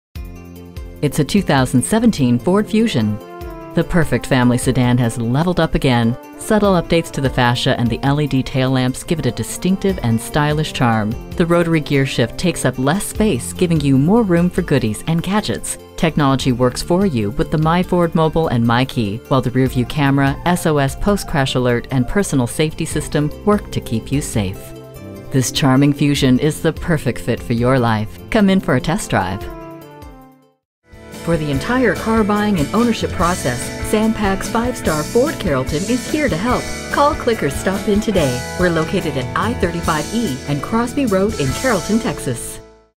New 2017 Ford Fusion Dallas Ft Worth DFW, TX
Year: 2017
Make: Ford
Model: Fusion
Trim: SE
Engine: 4 Cyl. 1.5 L
Transmission: Automatic
Color: Oxford White
Interior: Med Light Stone
Stock #: HR406598
VIN: 3FA6P0HD0HR406598
2017 Ford Fusion SE 34/23 Highway/City MPG

Address:
1635 Interstate 35